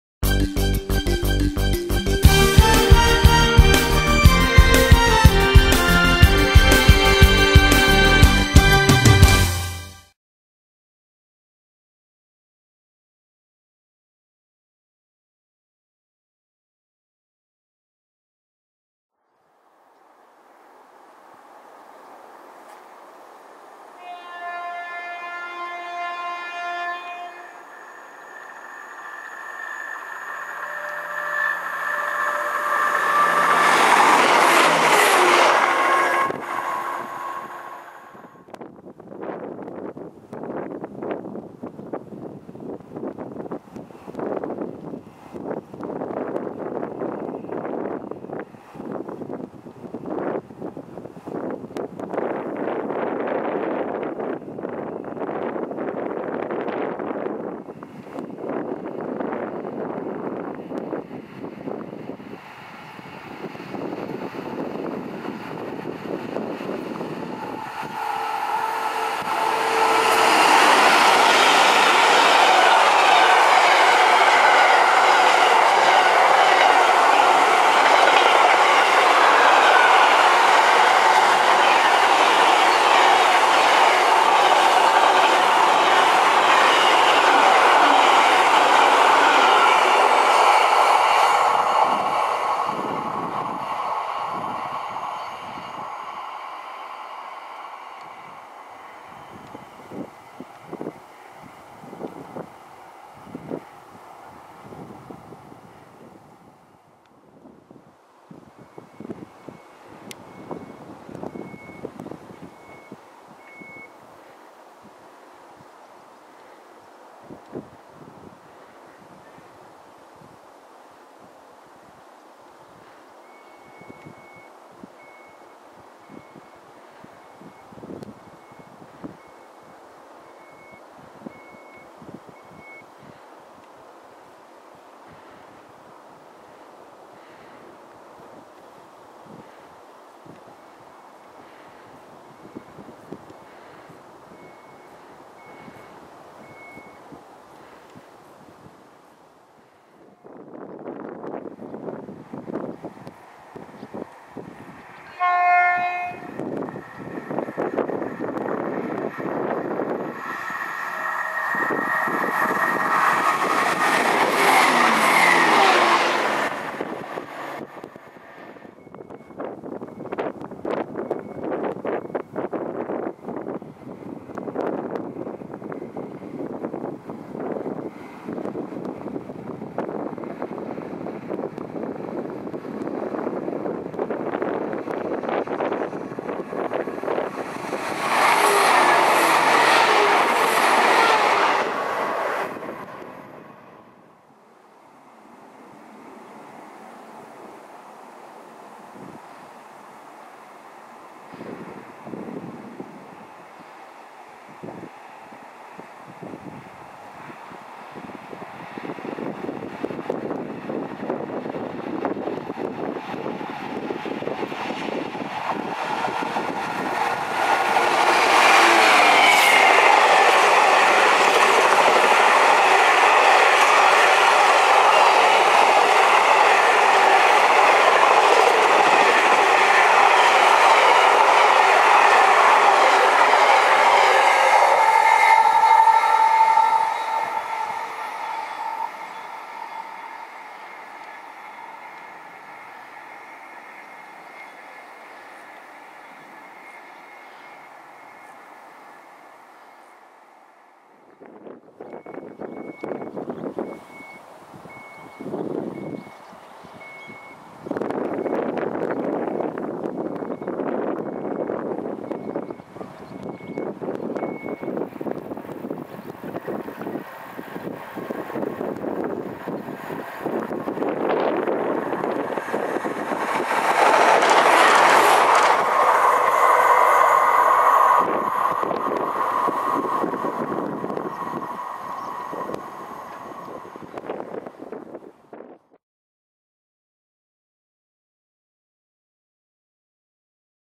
Birmingham to Peterborough Line Near Burbage 22.08.2013
Video from the Birmingham to Peterborough Line near Burbage on the 22nd August 2013.

Class 170's and Class 66's feature in the liveries of Cross Country Trains, EWS (now DB Schenker) and GBRf.

In order of appearance; GBRf Class 66's, No. 66722 'Sir Edward Watkin' (in Metronet livery) (leading) and No. 66703 'Doncaster PSB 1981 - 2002' double heading east with a Hams Hall to Felixstowe intermodal  and DB Schenker Class 66, No. 66132 heading east with a Birch Coppice - Felixstowe intermodal.